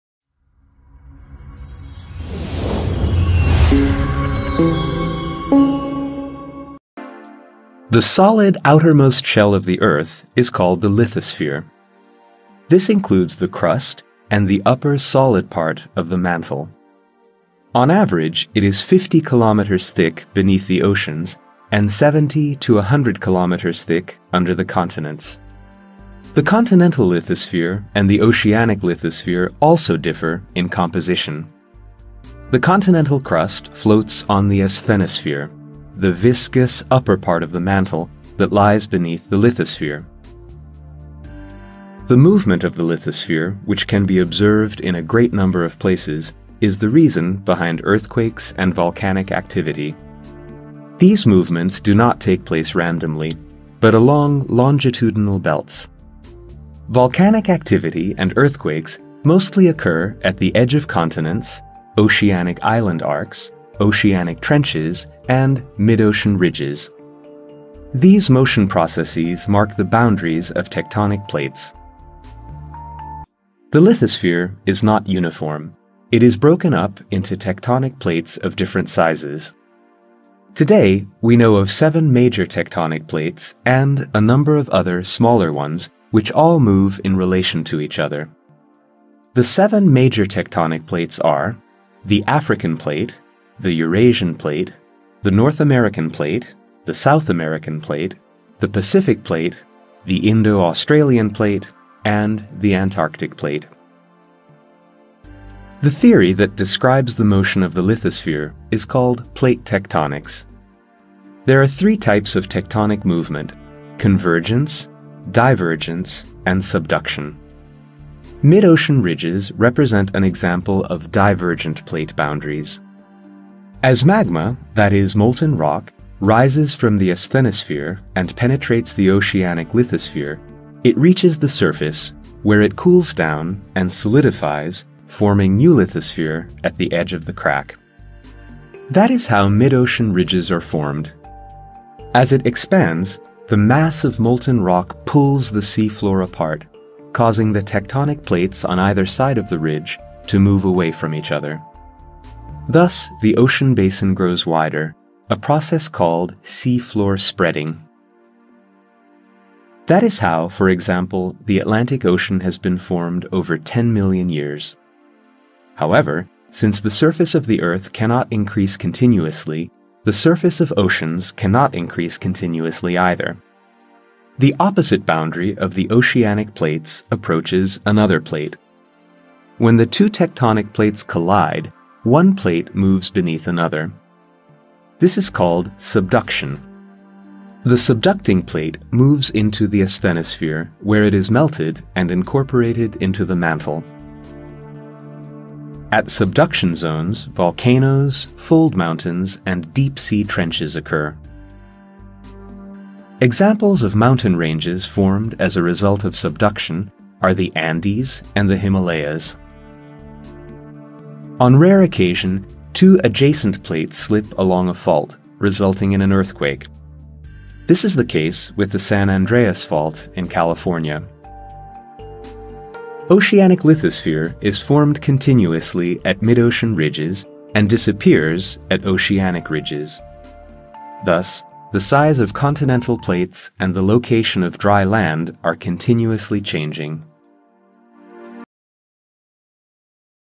Tectonic Plates, 3D Structure of the Earth
In this educational video, we explore the fascinating world of tectonic plates and the 3D structure of the Earth. You will learn about the different types of tectonic plates, how they move and interact with each other, and the various geological phenomena that result from these movements, such as earthquakes and volcanic eruptions. With our 3D visualization technology, you'll get an up-close look at the intricate layers and structures of our planet. Join us on this journey to discover the secrets of the Earth's dynamic surface.